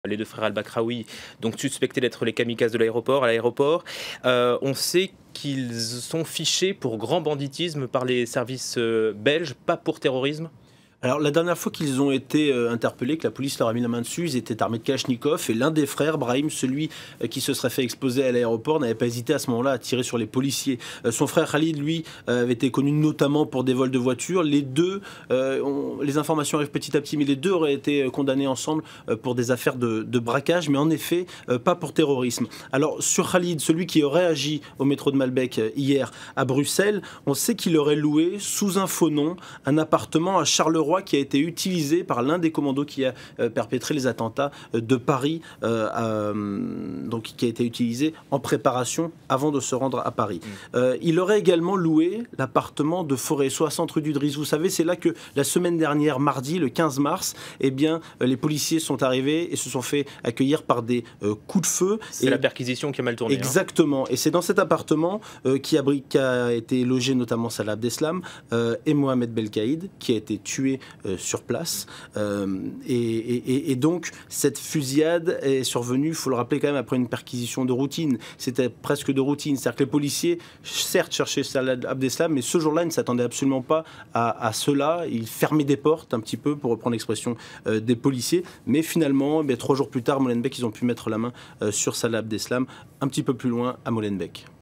Attentats de Bruxelles - Qui sont les frères El Bakraoui, kamikazes du groupe État islamique ?
Au lendemain des attentats de Bruxelles, les deux kamikazes présumés ont été identifiés comme étant deux frères, dont l'un était en contact avec Salah Abdeslam.